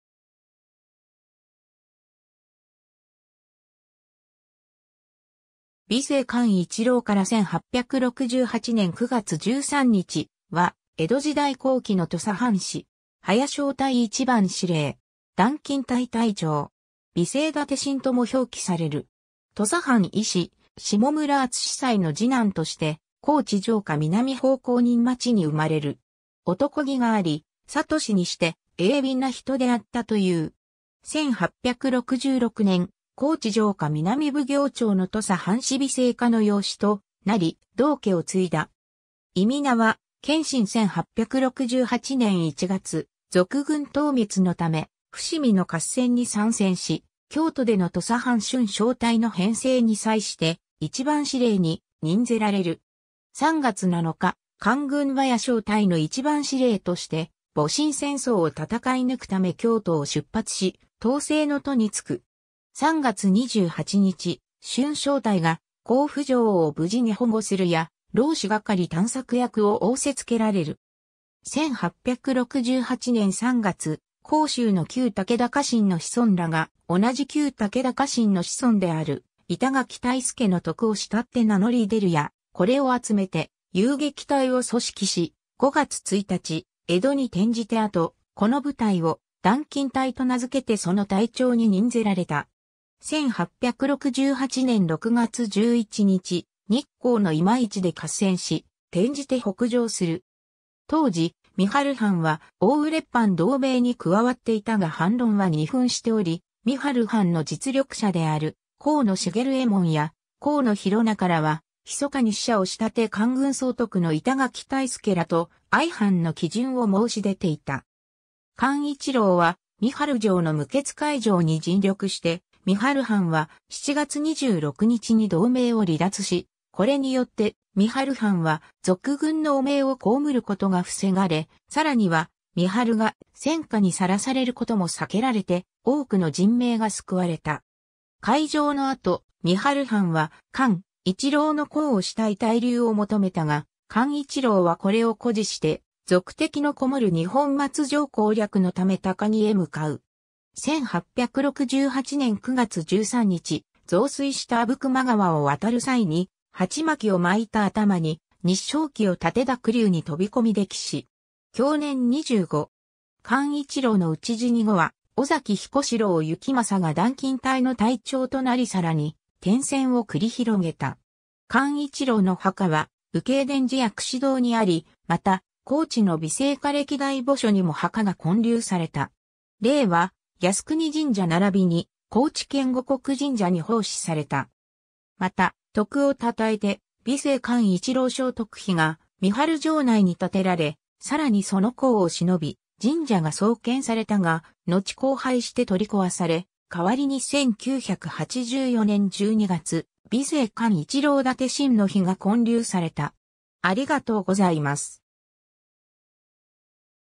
美正貫一郎
美正 貫一郎（みしょう かんいちろう、1844年（天保15年1月） - 1868年9月13日（慶応4年7月27日））は、江戸時代後期の土佐藩士。
迅衝隊一番司令。
断金隊隊長。
美正建臣とも表記される。
 土佐藩医師・下村惇斎の二男として高知城下南奉公人町に生まれる。
男気があり智にして鋭敏な人であったという。
 1866年（慶応2年）、高知城下南奉行町の土佐藩士（徒士格）美正家の養子となり同家を嗣いだ。
諱は「建臣」 1868年（慶応4年）1月、賊軍討滅の為、伏見の合戦に参戦し、京都での土佐藩迅衝隊の編成に際して、一番司令に任ぜられる。
 3月7日（2月14日）、官軍迅衝隊の一番司令として戊辰戦争を戦い抜くため京都を出発し東征の途につく。
 3月28日（3月5日）、迅衝隊が甲府城を無事に保護するや、「浪士掛探索役」を仰せ付けられる。
 1868年（慶応4年）3月、甲州の旧武田家臣の子孫らが、同じ旧武田家臣の子孫である板垣退助の徳を慕って名乗り出るや、これを聚めて遊撃隊を組織し、5月1日（4月9日）、江戸に転じて後、この部隊（総勢約150人）を断金隊と名付けてその隊長に任ぜられた。
 1868年6月11日（閏4月21日）、日光の今市で合戦し、転じて北上する。
 当時、三春藩は奥羽列藩同盟に加っていたが藩論は二分しており、三春藩の実力者である河野卯右衛門や、河野広中（信次郎）らは、密かに使者を仕立て官軍総督の板垣退助らと会い藩の帰順を申し出ていた。
貫一郎は、三春城の無血開城に尽力して、三春藩は、7月26日に同盟を離脱し、これによって三春藩は賊軍の汚名を蒙ることが防がれ、さらには三春が戦火に晒されることも避けられて多くの人命が救われた。
開城の後、三春藩は貫一郎の功を慕い滞留を求めたが、貫一郎はこれを固辞して賊敵の籠もる二本松城攻略のため高木（福島県本宮市）へ向かう。
 1868年9月13日（慶応4年7月27日）、増水した阿武隈川を渡る際に、鉢巻を巻いた頭に日章旗を立て濁流に飛び込み溺死。
享年25。
 貫一郎の討死後は、尾崎彦四郎行正（尾崎行雄の父）が断金隊の隊長となりさらに転戦を繰り広げた。
 貫一郎の墓は誓伝寺薬師堂（所在地 福島県本宮市本宮上町155）にあり、また高知の美正家歴代墓所にも墓が建立された。
霊は靖国神社ならびに高知縣護國神社に奉祀された。
また、徳を讃えて「美正貫一郎頌徳碑」が三春城内に建てられ、さらにその功を偲び、神社が創建されたが、のち荒廃して取り壊され、代わりに1984年（昭和59年）12月、「美正貫一郎建臣之碑」が建立された。